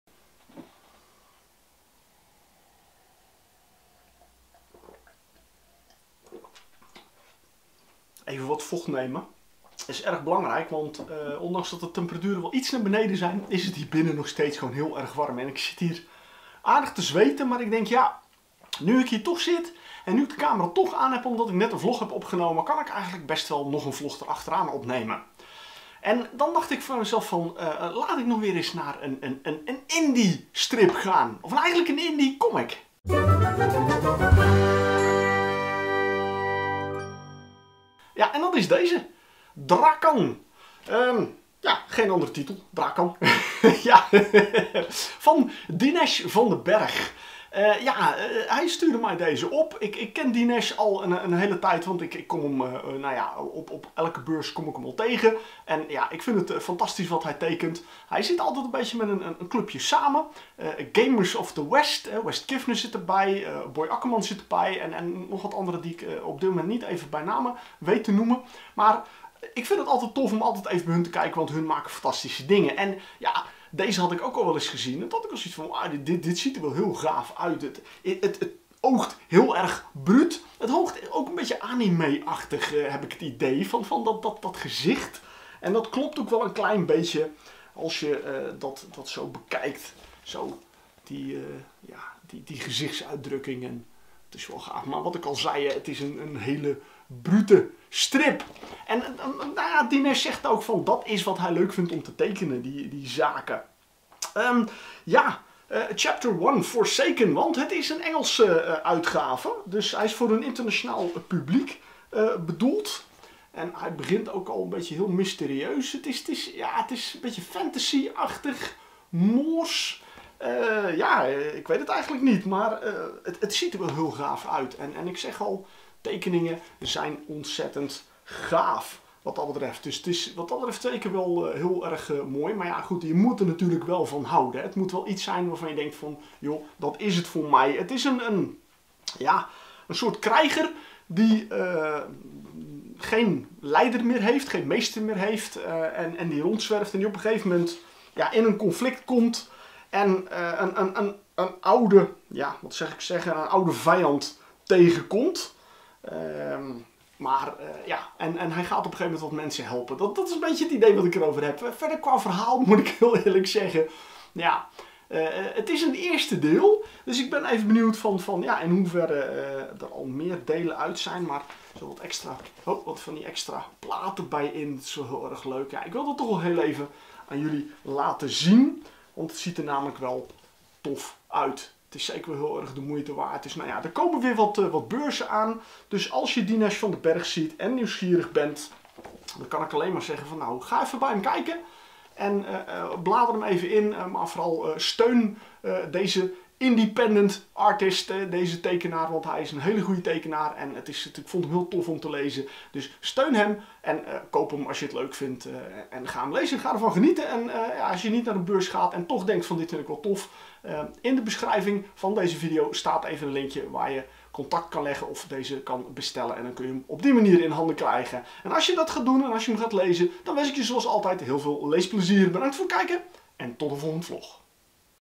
Smallpress vlog: Drakan
Tijd voor smalpress, een indie comic. Drakan is een comic van Dinesh van den Berg. Ik kom hem vaak tegen op beurzen en hij maakt toffe dingen, daarom vind ik het leuk om een vlog te kunnen brengen over zijn werk.

Vind je mijn video's leuk en wil je graag een steentje bijdragen, dat kan! Je kan me trakteren op een kop koffie

Copyright alle artwork bij de respectievelijke eigenaren